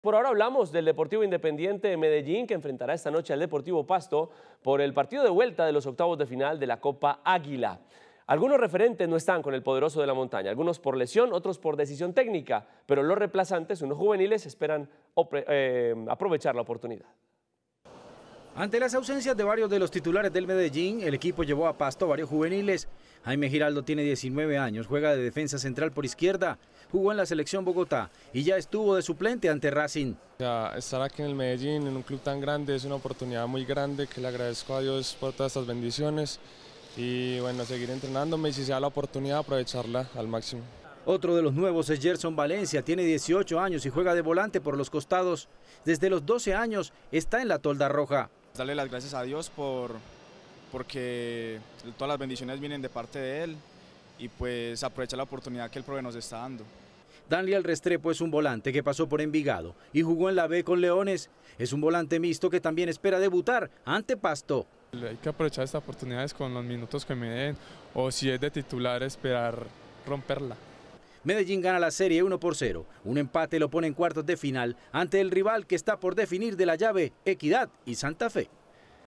Con juveniles en su nomina, DIM espera traer la clasificación de Pasto[Noticias] - Telemedellín
El DIM jugará esta noche como visitante el partido de ida por octavos de final de Copa Aguila ante el Deportivo Pasto.